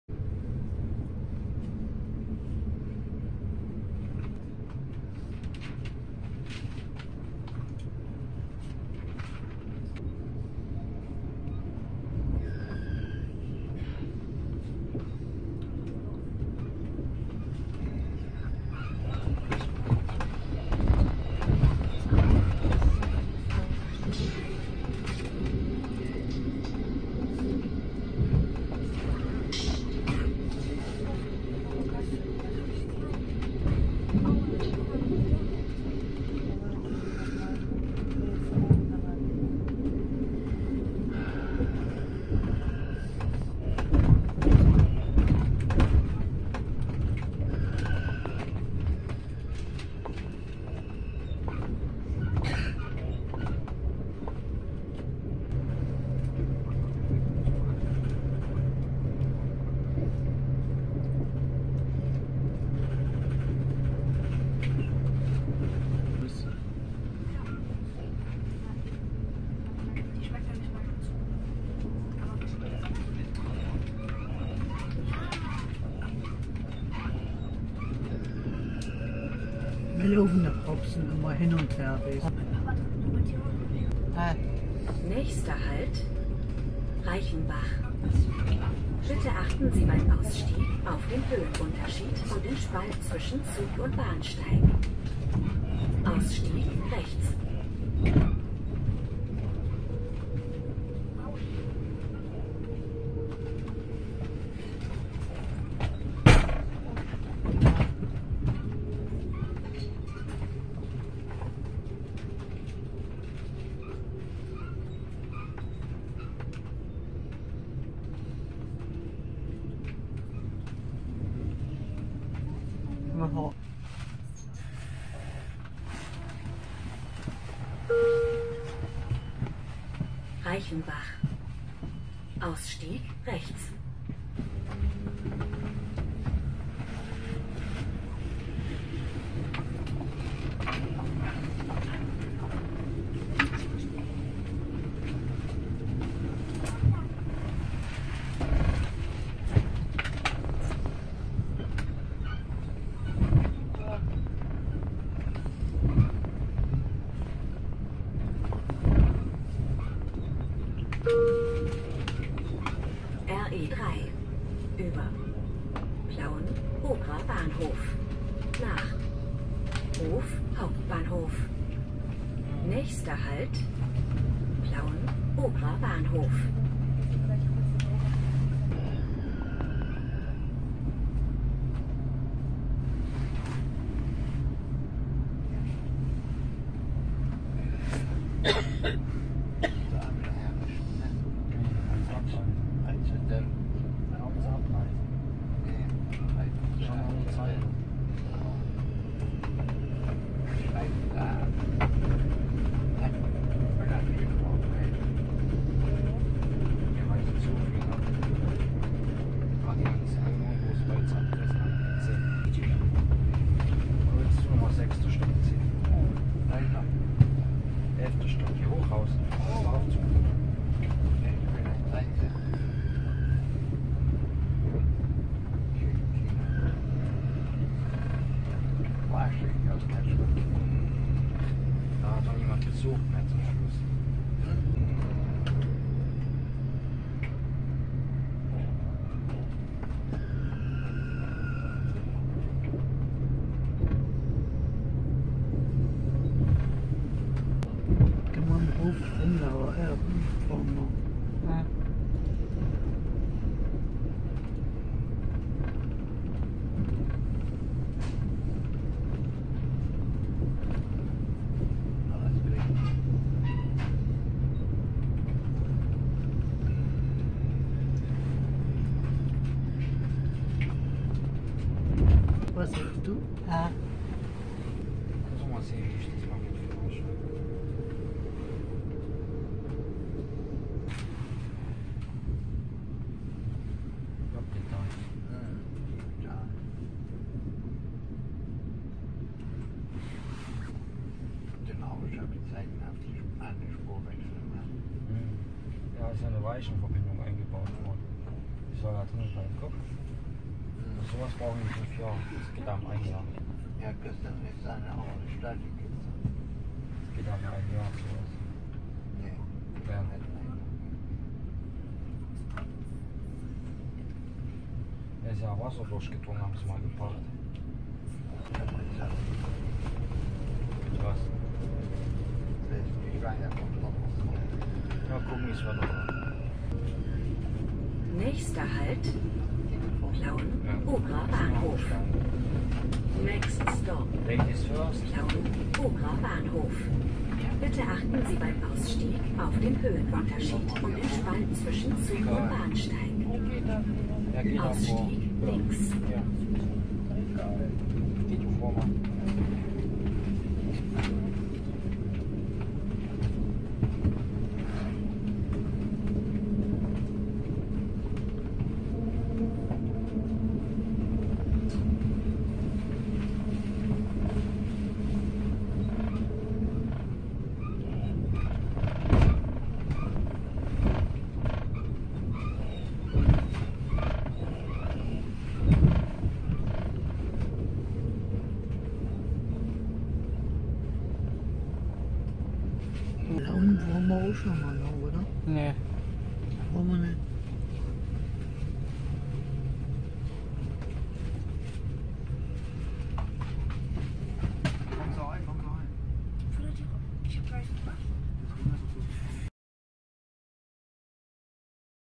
Die Mitfahrt im re3 von Dresden hbf nach Hof Hbf über Chemnitz Hbf, Zwickau hbf (T.4) in Sachsen.
Die Mitfahrt im re3 von Dresden hbf nach Hof Hbf über Chemnitz Hbf, Zwickau hbf (T.4) in Sachsen am 08.04.23.